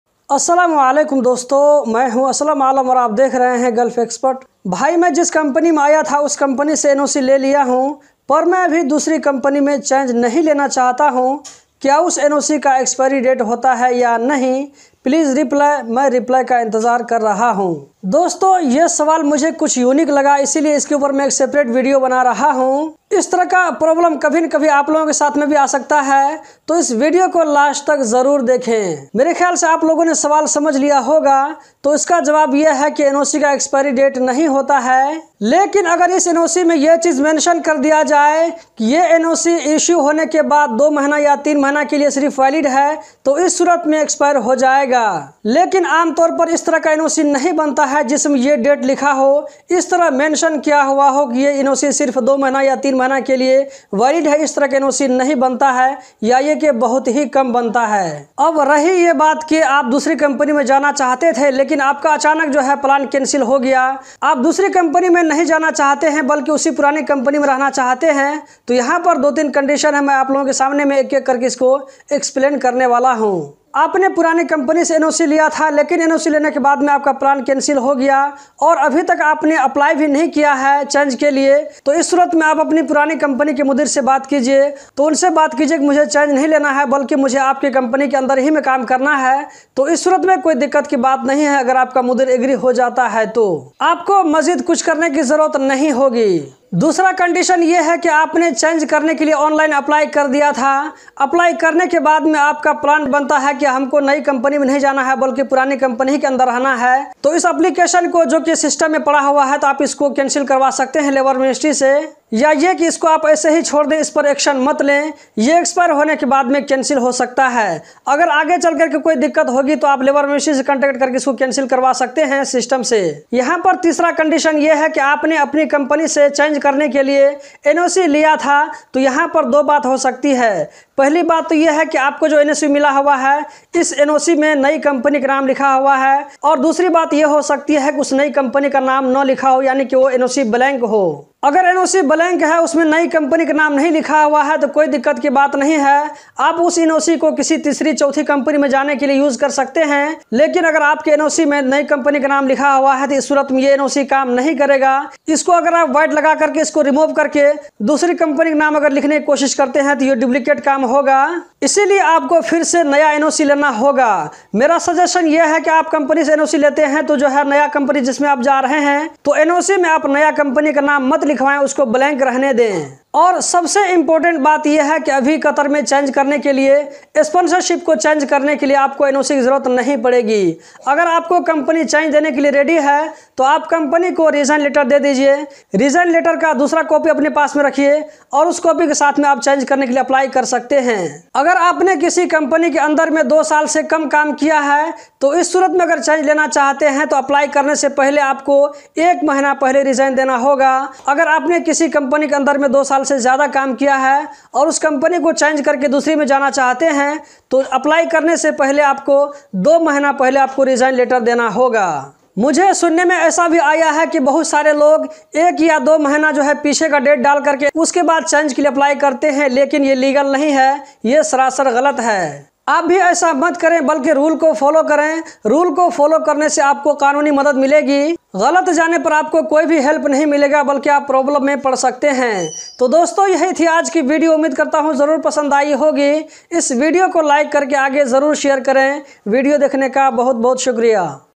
Doha Qatar | NOC और कफाला चेंज के बारे में एक अहम् जानकारी | Sponsorship Change & NOC in Qatar
Doha Qatar | NOC और  कफाला चेंज के बारे में अहम् जानकारी | Sponsorship Change & NOC in Qatar